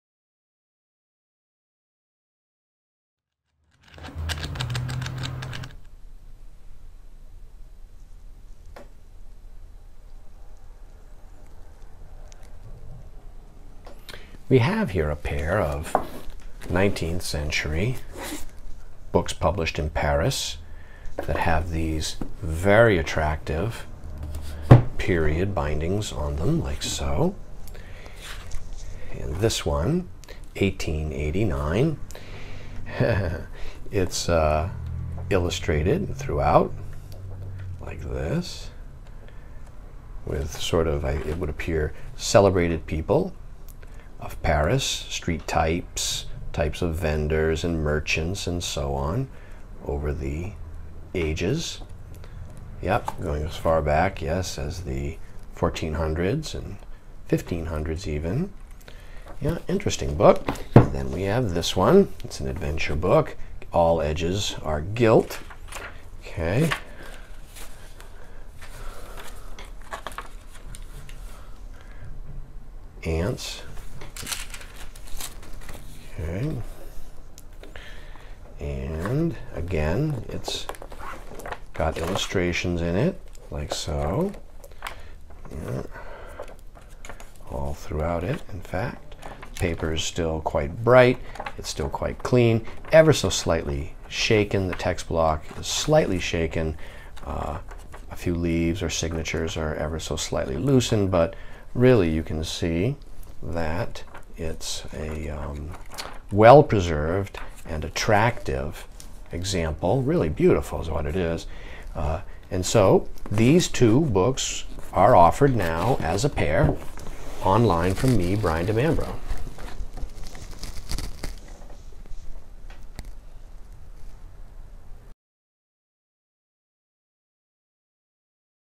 Ant Fable & Paris street vendors 1880's French decorative lot 2 old books illustrated
Short video captures condition much better than words. Please watch prior to purchase to ensure a clear understanding of the pair.

[French books- 19th century decorative bindings].

Les Cris de Paris: Types et Physionomes D'Autrefois, by Victor Fournel.
Publised 1889, Paris by Firmin-Didot et Cie.
221 pages. Depicts the historical range of street vendors in paris through the ages.

Les Aventures d'une Fourmi Rogue et Les Memoires d'un Pierrot, by H. de La Blanchere. Illustration de Mesnel et de Giacomelli.
Published c. 1880, Paris, by Theodore Lefevre.
266 pages. Curious story involving ants.

Two lovely 19th century French books, in period decorative gilt bindings. Both well illustrated throughout.

Both very clean and attractive condition, pleasing books as objects.

Set along the shelf measures: 2 1/2".
Largest book measures: 9 1/2" H X 6 1/4" W.
Smallest book measures: 8 3/4" H X 5 3/4" W.

[V1763].